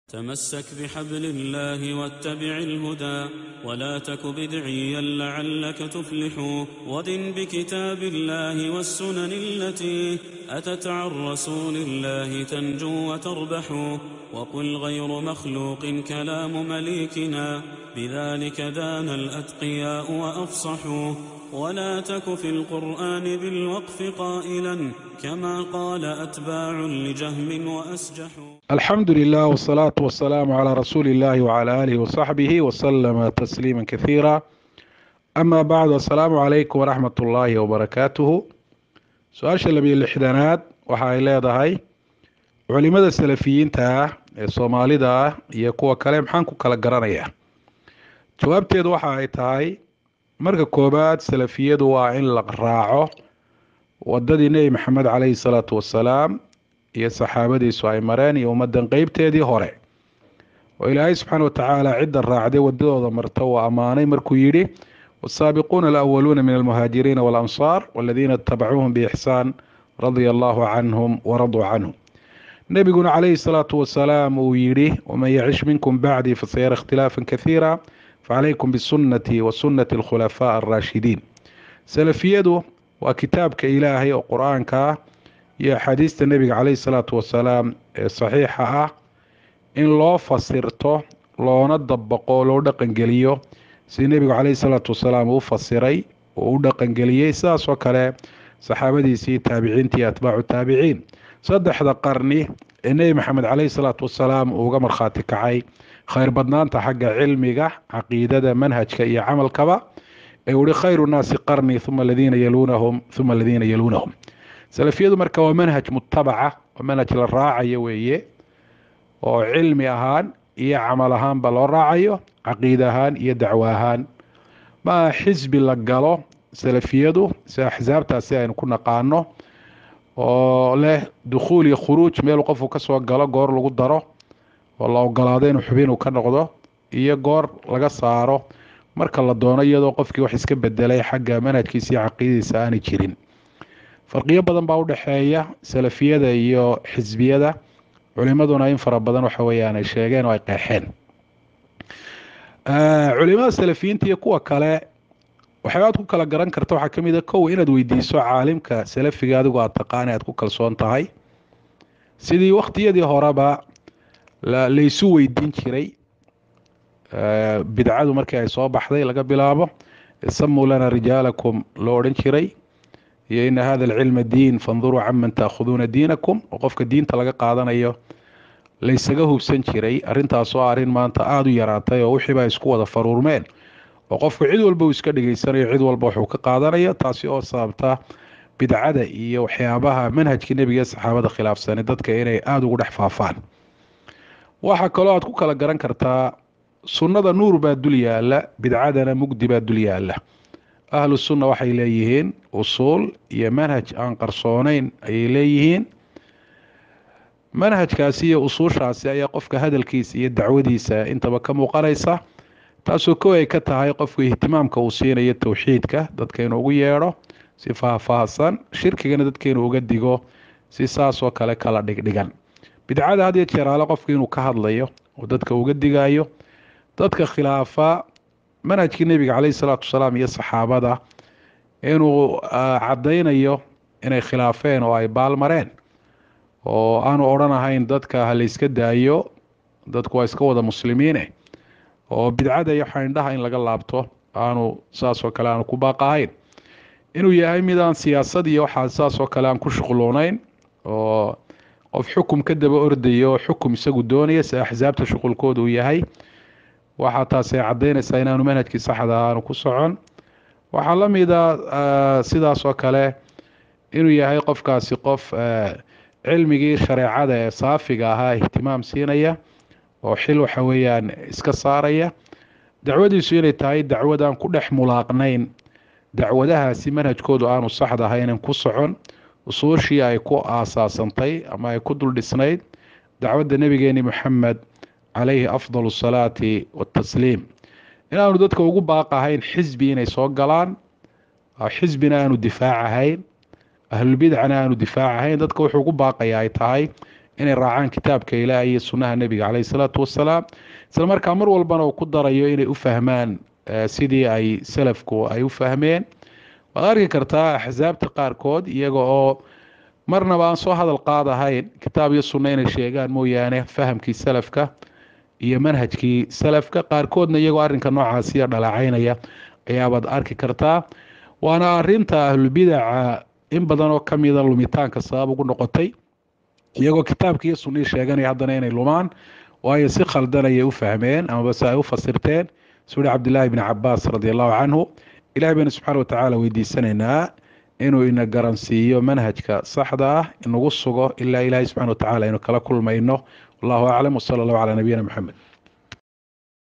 Su'aalaha Whatsappka ||Su'aasha 62aad|| الأسئلة والأجوبة || Sheekh Cabdilaahi Barbaraawi حفظه الله
Su'aasha 62aad:
Culimada Salafiyiinta Ah Ee Soomaaliyeed iyo Kuwa Kale Maxaan Ku Kala Garanayaa?